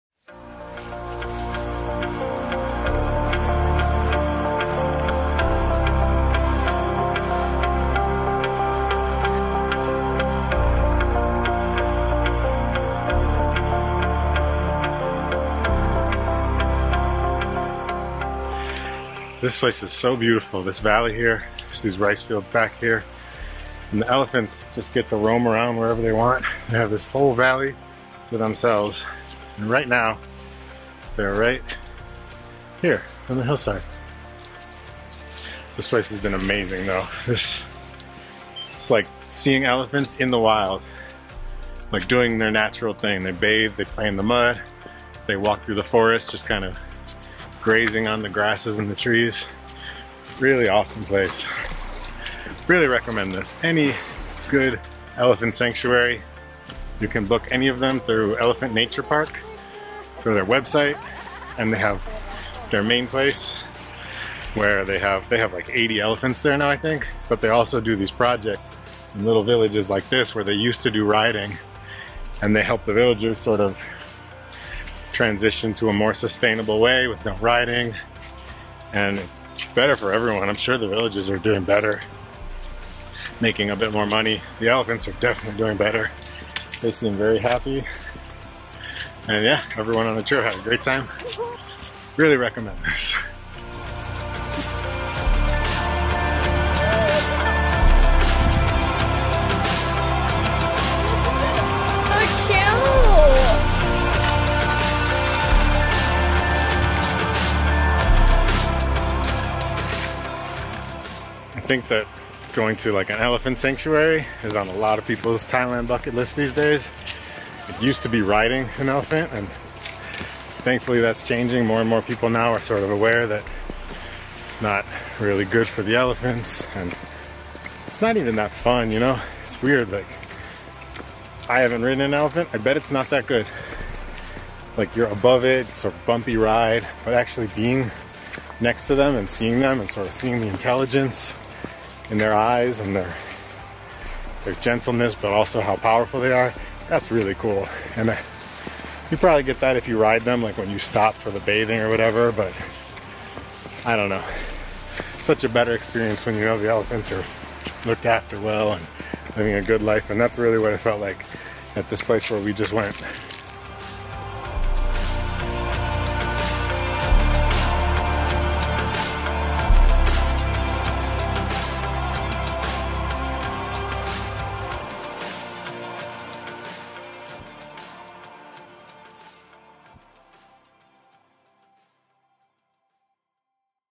Thailand ethical elephant sanctuary + adorable 6 week old baby!
Elephant tourism is super popular in Thailand but booking an actually ethical experience can be difficult. Here's exactly how to do it, plus a few minutes of our experience with these incredible creatures.

For full details on how to book through the Elephant Nature Park, or to find their partner projects in Phuket, Samui, and Pattaya, check out our full guide to booking ethical elephant experiences on our website